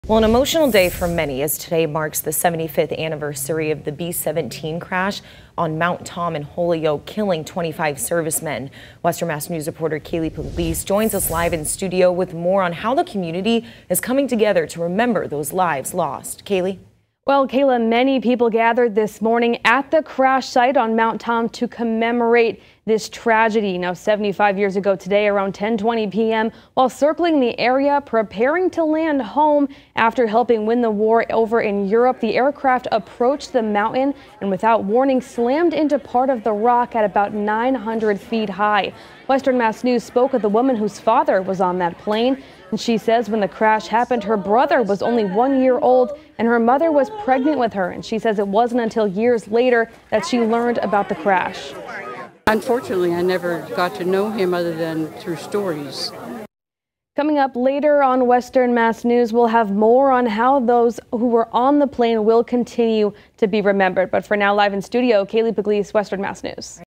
Community comes together to remember the 75th anniversary of the Holyoke B17 crash
75 years later, dozens gathered at the site of the crash Saturday morning to remember those lives lost.